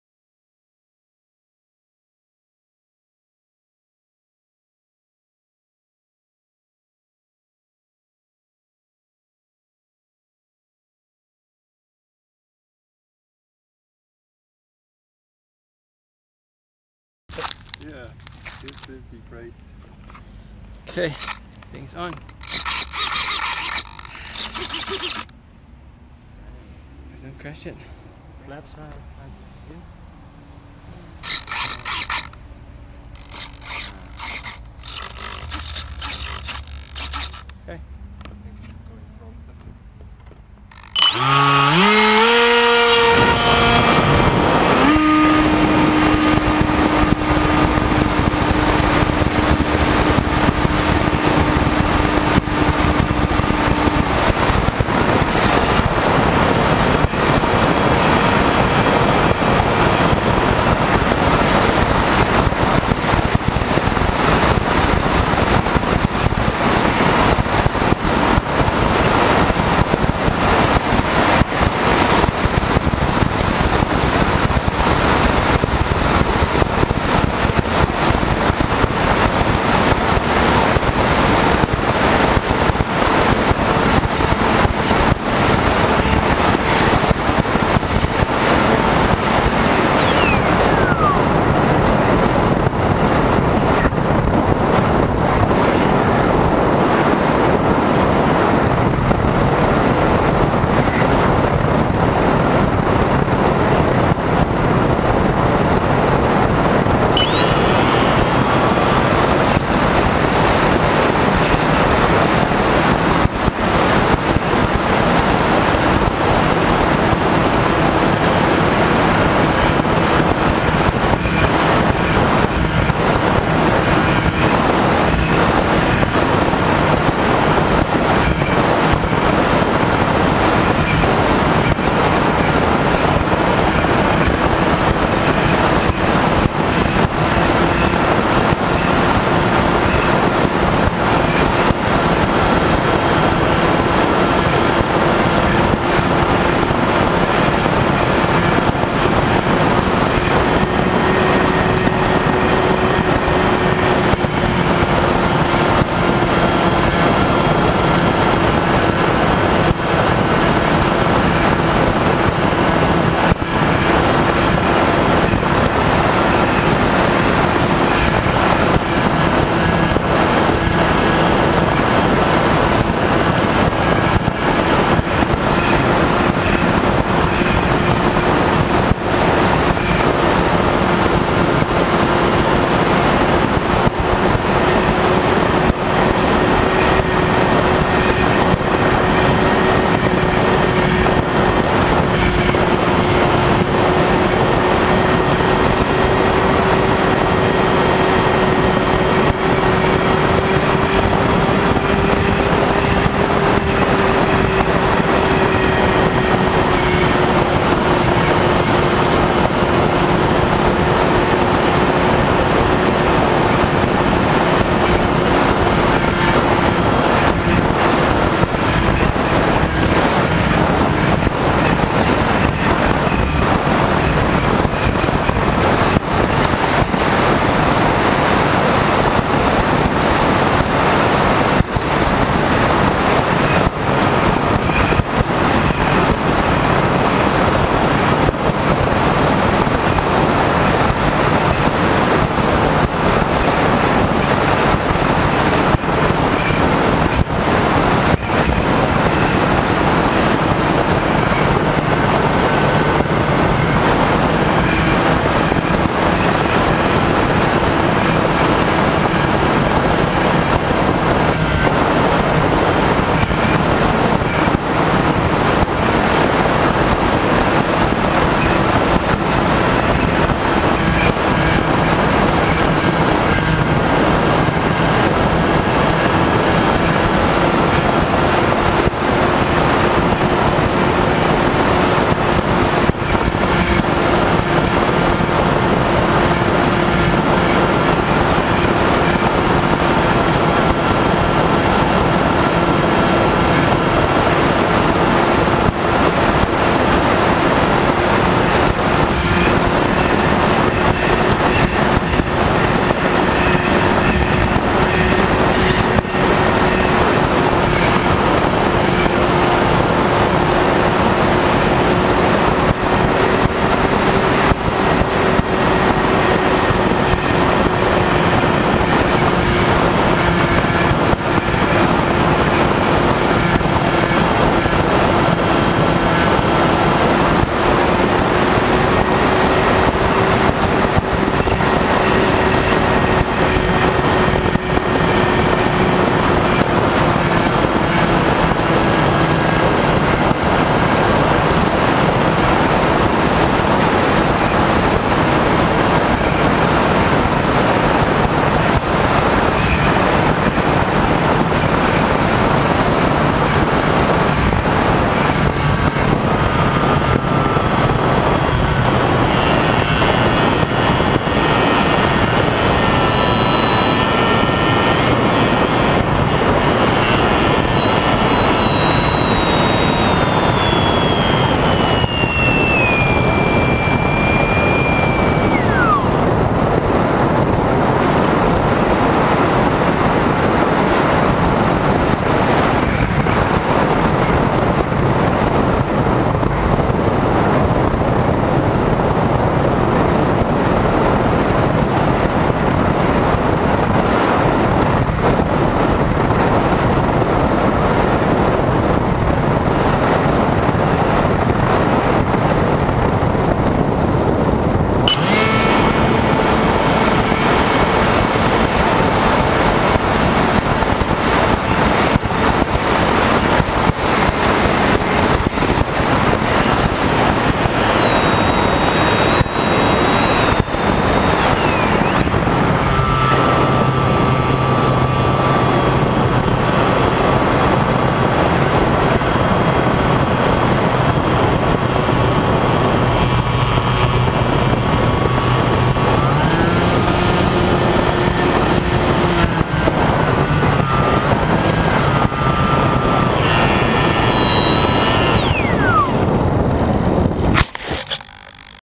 HK Bixler with Ardupilot
HK Bixler Glider Plane with Ardupilot autopilot
Cameras: GoPro & Turniy keychain
Radio system: Turnigy 9X
Battery: Turnigy 1600 mAh 11.1 V 3S 20-30C
Flying altitude: 80m
Location: Binz, Zurich, Switzerland
Weather: Great! Moderate wind gusts